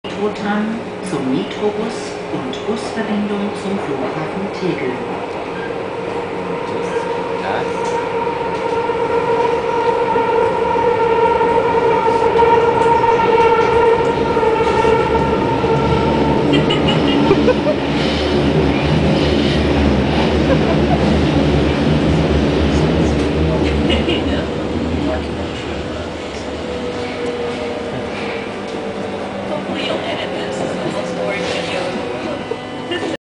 Susy and Anders
Susy and Anders on their last night in Berlin - boring video?  I think not.